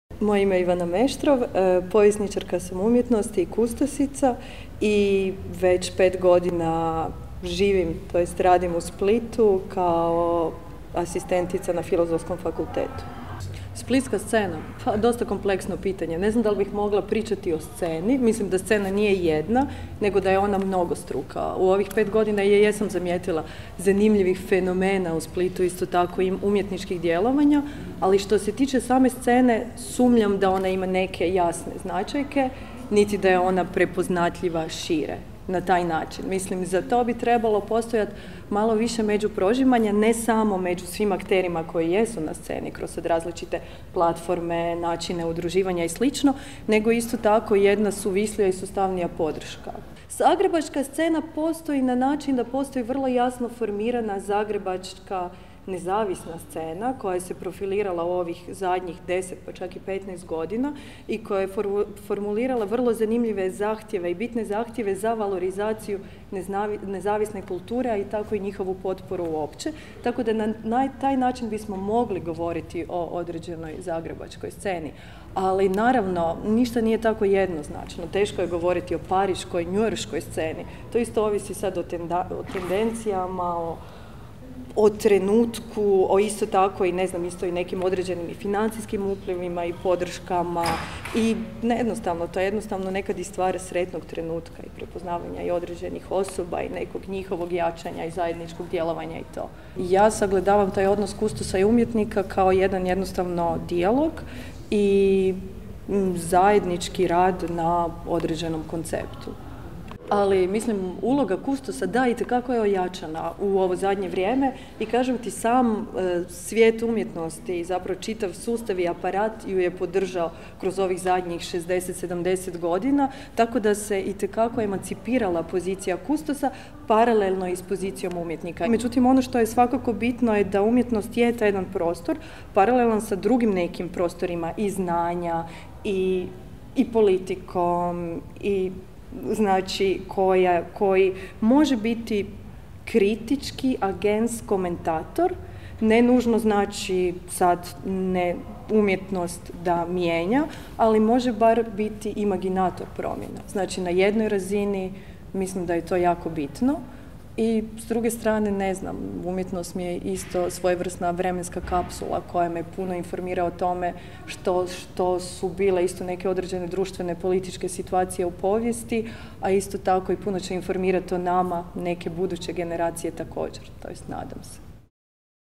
Ivana Meštrov - Splitska Umjetnička Scena
Kako bi se sagledalo mišljenje umjetnika i kustosa koji žive i djeluju u Splitu i bližem okružju, o dvije temeljne problematike vezane za njihov rad i njegovu vidljivost u široj društvenoj sferi kao i načinima zajedničkih i sinergijskih djelovanja, intervjuiran je velik broj njih te zatražen osvrt i njihova neposredna opservacija:
- što mislite o suvremenoj umjetničkoj sceni u Splitu, kako se artikulira, kakva su strujanja i usmjerenja na njoj, uočavate li određene polarizacije i je li uopće moguće tako pristupiti situaciji i sagledavati je na taj način, je li moguće promatrati termin "scena" kao monolitan, postoji li takvo što kao umjetnička scena u Splitu...?
-kako percipirate poziciju umjetnika u društvu danas, je li umjetnik može biti agent promjena, kakva je moć umjetnosti koju producira...?
Kao prilog 39. splitskom salonu napravljen je video uradak "Abeceda" u kojem je intervjuirano stotinjak predstavnika splitske umjetničke scene.

Kustosi 39. splitskog salona: Božo Kesić, Dalibor Prančević, Boris Šitum
Snimatelj: Ivan Perić
Montaža: Boris Šitum, Ivan Perić, Marko Jukić
Obrada slike i zvuka: Dragan Kovačević
Za organizatora: Vice Tomasović
Organizacija: Hrvatska udruga likovnih umjetnika - Split